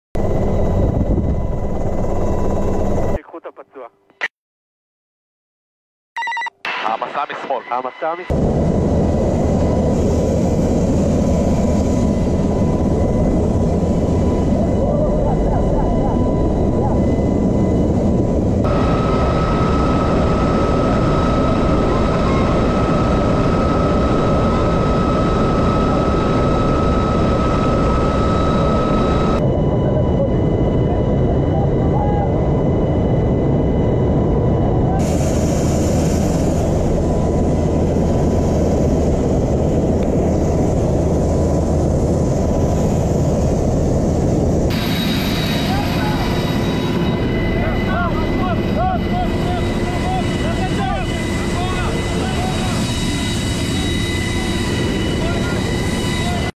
Resgate feridos em GAZA IAF (ISRAEL AIR FORCE)
669 unidades resgatam feridos de Gaza.

Desde o início da guerra, a unidade, juntamente com outras unidades, tem operado em Gaza sob fogo constante, resgatando e tratando soldados das FDI.

Até agora, realizaram 150 operações terrestres e de resgate, evacuando 260 soldados feridos para hospitais.